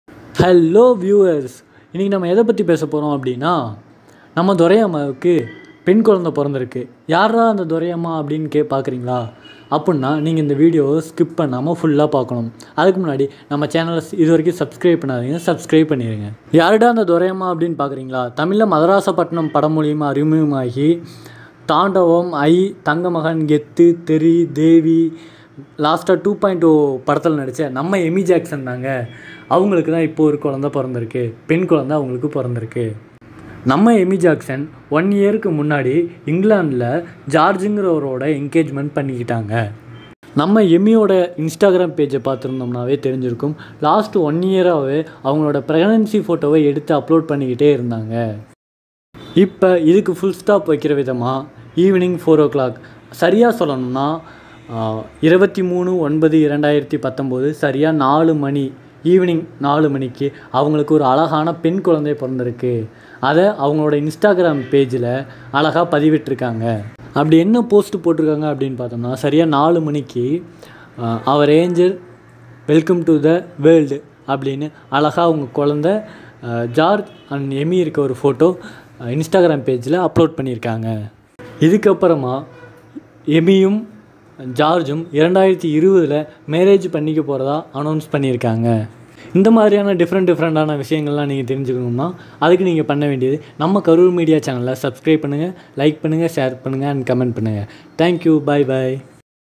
Amy jackson Praised Baby Girl | 2.0 Heroine
Actress Amy jackson Praised baby girl today. This happy news updated for Amy's Instagram Page.